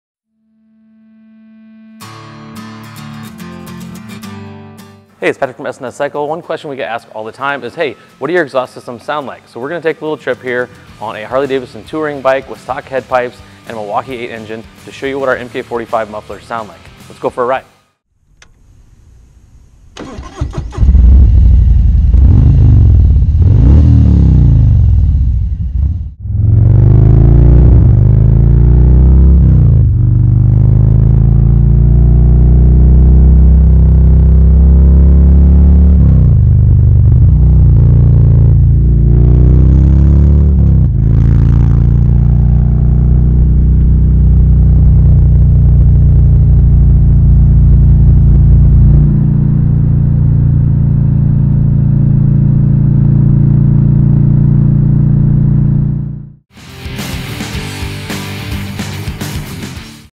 S&S Cycle - MK45 Mufflers for M8 Equipped Baggers - On Board Sound
Listen to the deep rumble of our MK45 mufflers on a Harley Davidson touring bike equipped with the H-D® Milwaukee Eight® engine.